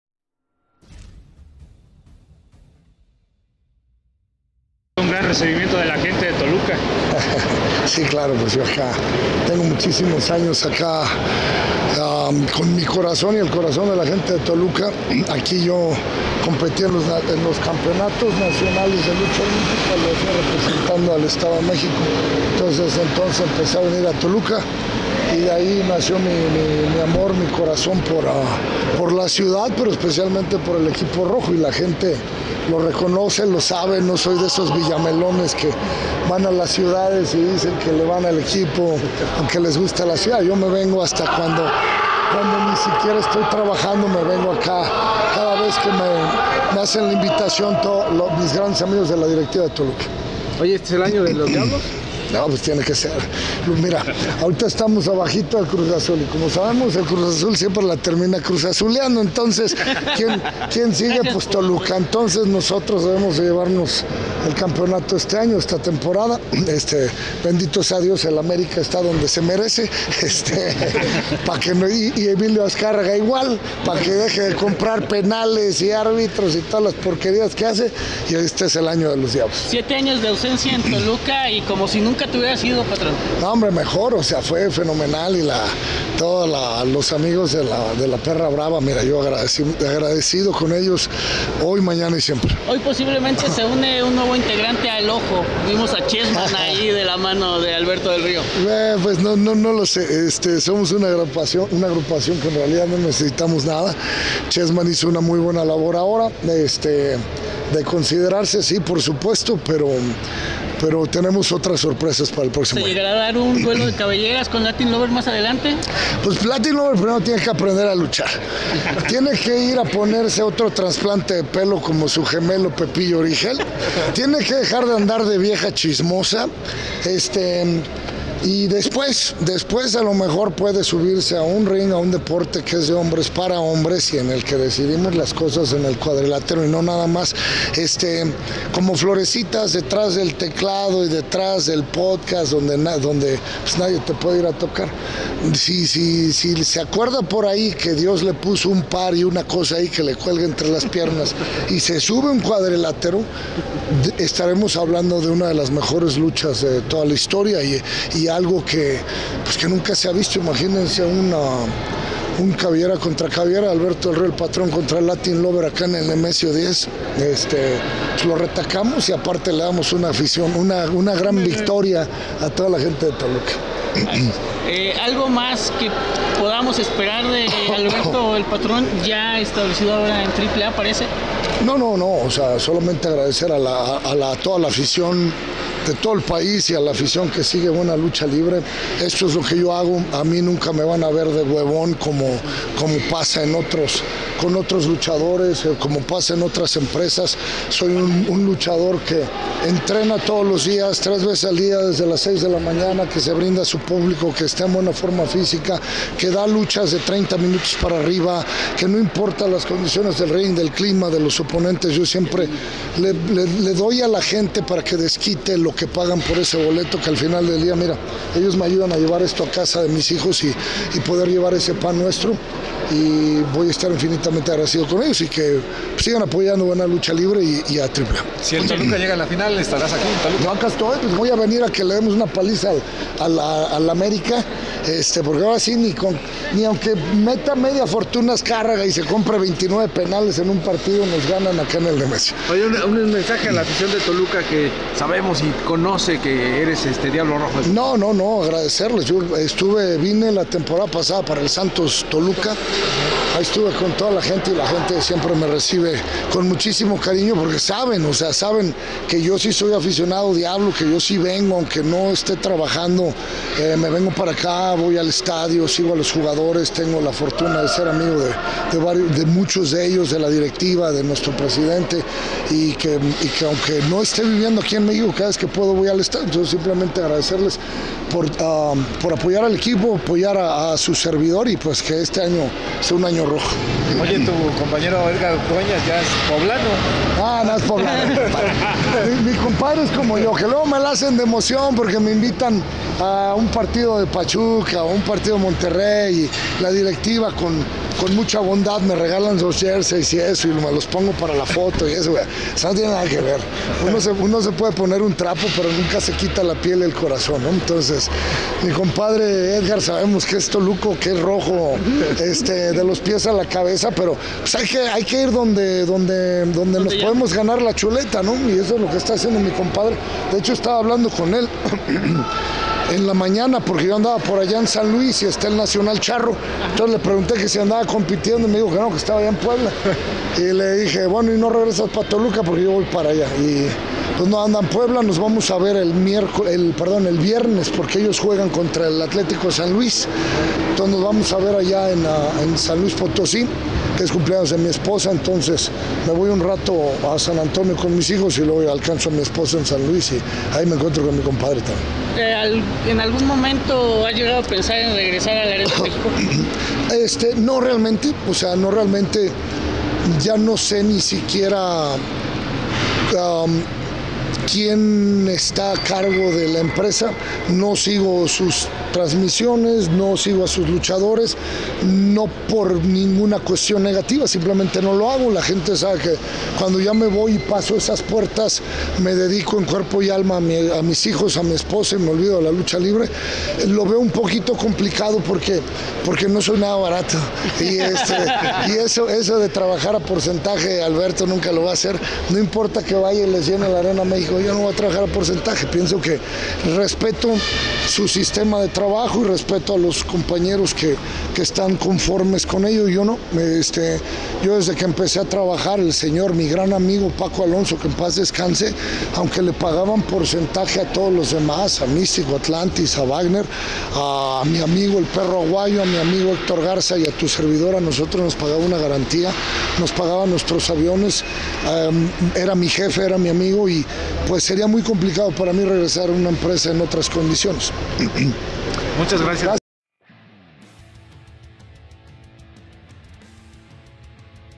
ENTREVISTA CON ALBERTO DEL RIO
ALBERTO DEL RIO NOS CUENTA DE SUS SENSACIONES TRAS 7 AÑOS DE AUSENCIA EN TOLUCA, ¿REGRESARIA A LA ARENA MEXICO?, ¿TOLUCA SERA CAMPEON?, ¿VEREMOS CABELLERA VS CABELLERA ANTE LATIN LOVER? AQUI NOS CUENTA TODO Y MUCHO MAS